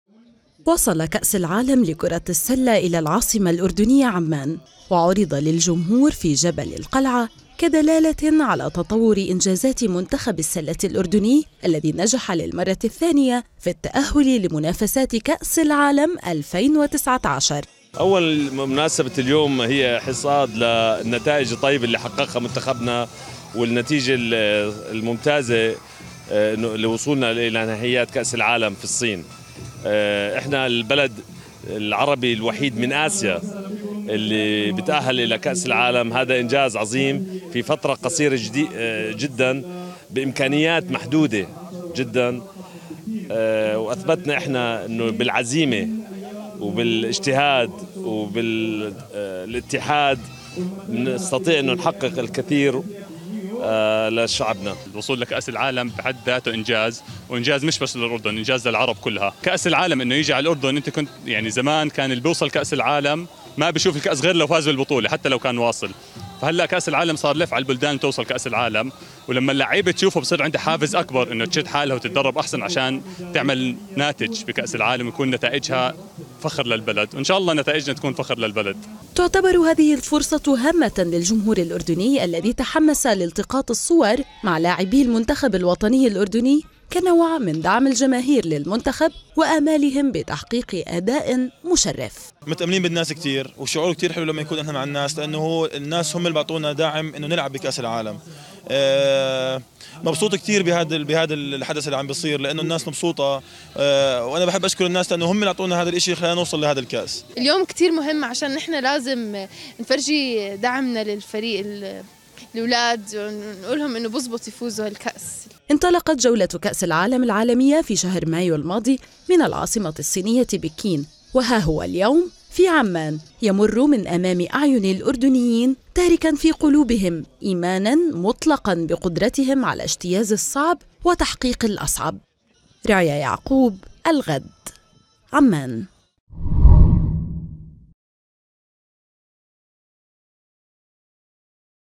كأس العالم لكرة السلة تصل الأردن
زار كأس_العالم لكرة السلة في العاصمة الاردنية عمان خلال جولة بدأت من برلين ، وتجمع عدد من الأردنيين لأخذ الصور مع الكأس واعضاء فريق المنتخب الأردني المتأهل لكأس العالم. وكانت القرعة قد اوقعت المنتخب الوطني الأردني في المجموعة السابعة الى جانب فرنسا وألمانيا وجمهورية الدومينيكان.. مزيد من التفاصيل في تقرير يارا يعقوب مراسلة الغد من عمان.